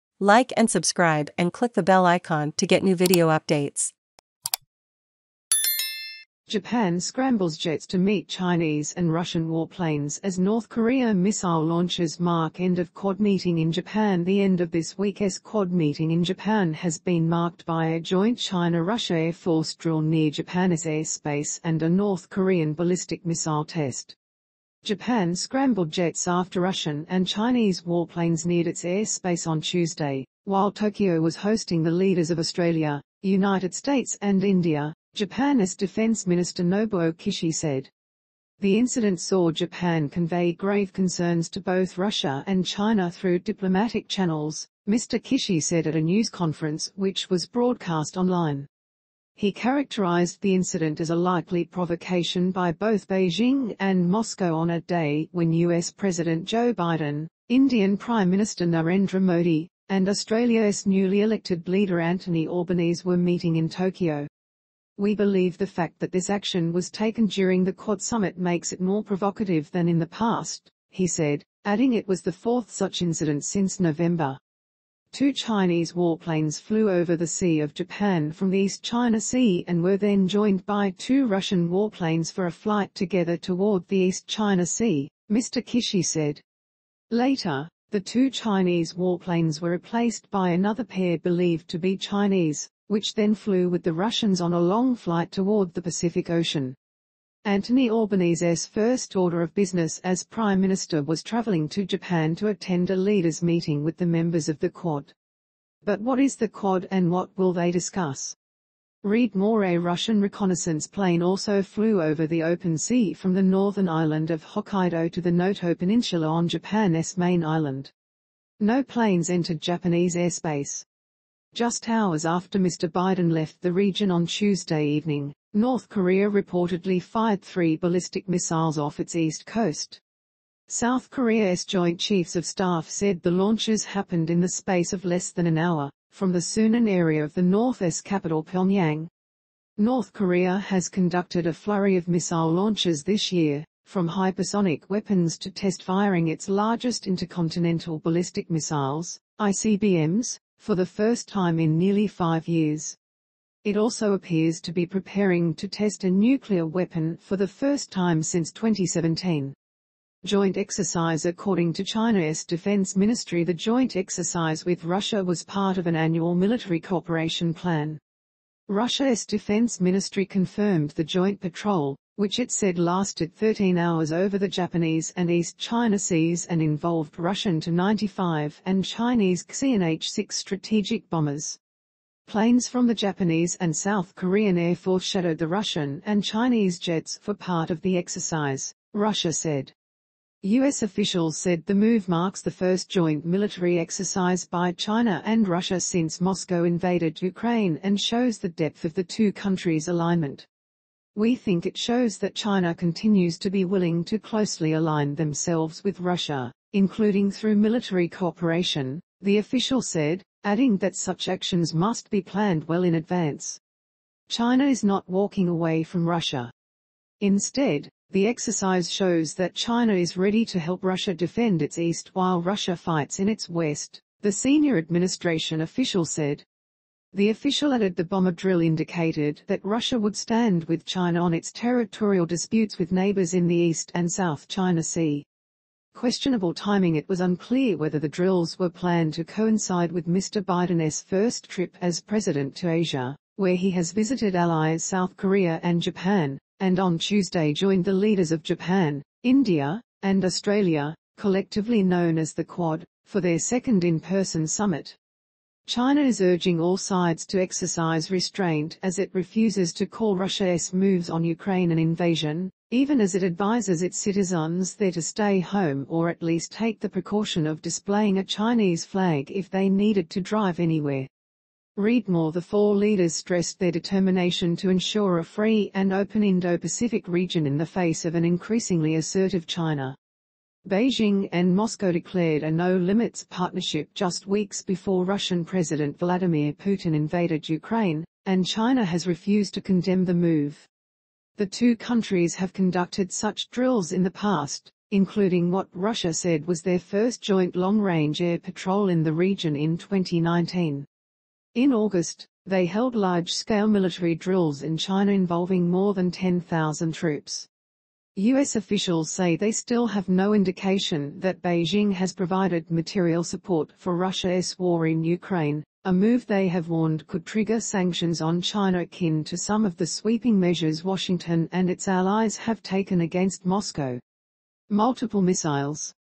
JAPAN SCRAMBLES JETS TO MEET CHINESE AND RUSSIAN WARPLANES AS NORTH KOREA MISSILE...
Japan scrambles jets to meet Chinese and Russian warplanes as North Korea missile launches mark end of Quad meeting in Japan

The end of this week's Quad meeting in Japan has been marked by a joint China Russia air force drill near Japan's airspace and a North Korean ballistic missile test. Japan scrambled jets after Russian and Chinese warplanes neared its airspace on Tuesday. while Tokyo was hosting the leaders of Australia. United States and India. Japan's Defence Minister Nobuo Kishi said. The incident saw Japan convey 'grave concerns' to both Russia and China through diplomatic channels. Mr Kishi said at a news conference which was broadcast online. He characterised the incident as a likely provocation by both Beijing and Moscow on a day when US President Joe Biden. Indian Prime Minister Narendra Modi. and Australia's newly elected leader Anthony Albanese were meeting in Tokyo. 'We believe the fact that this action was taken during the Quad summit makes it more provocative than in the past.' he said. adding it was the fourth such incident since November. Two Chinese warplanes flew over the Sea of Japan from the East China Sea and were then joined by two Russian warplanes for a flight together toward the East China Sea. Mr Kishi said. Later. the two Chinese warplanes were replaced by another pair believed to be Chinese. which then flew with the Russians on a long flight toward the Pacific Ocean. Anthony Albanese's first order of business as Prime Minister was travelling to Japan to attend a leaders' meeting with the members of the Quad. But what is the Quad and what will they discuss? Read more A Russian reconnaissance plane also flew over the open sea from the northern island of Hokkaido to the Noto peninsula on Japan's main island. No planes entered Japanese airspace. Just hours after Mr Biden left the region on Tuesday evening. North Korea reportedly fired three ballistic missiles off its east coast. South Korea's Joint Chiefs of Staff said the launches happened in the space of less than an hour. from the Sunan area of the North's capital Pyongyang. North Korea has conducted a flurry of missile launches this year. from hypersonic weapons to test firing its largest intercontinental ballistic missiles (ICBMs) for the first time in nearly five years. It also appears to be preparing to test a nuclear weapon for the first time since 2017. Joint exercise According to China's Defence Ministry the joint exercise with Russia was part of an 'annual military cooperation plan'. Russia's Defence Ministry confirmed the joint patrol. which it said lasted 13 hours over the Japanese and East China seas and involved Russian Tu 95 and Chinese Xian H 6 strategic bombers. Planes from the Japanese and South Korean air force shadowed the Russian and Chinese jets for part of the exercise. Russia said. US officials said the move marks the first joint military exercise by China and Russia since Moscow invaded Ukraine and shows the depth of the two countries' alignment. 'We think it shows that China continues to be willing to closely align themselves with Russia. including through military cooperation.' the official said. adding that such actions must be planned well in advance. 'China is not walking away from Russia. Instead. the exercise shows that China is ready to help Russia defend its east while Russia fights in its west.' the senior administration official said. The official added the bomber drill indicated that Russia would stand with China on its territorial disputes with neighbours in the East and South China Sea. Questionable timing It was unclear whether the drills were planned to coincide with Mr Biden's first trip as President to Asia. where he has visited allies South Korea and Japan. and on Tuesday joined the leaders of Japan. India. and Australia – collectively known as the Quad – for their second in person summit. China is urging all sides to exercise restraint as it refuses to call Russia's moves on Ukraine an 'invasion'. even as it advises its citizens there to stay home or at least take the precaution of displaying a...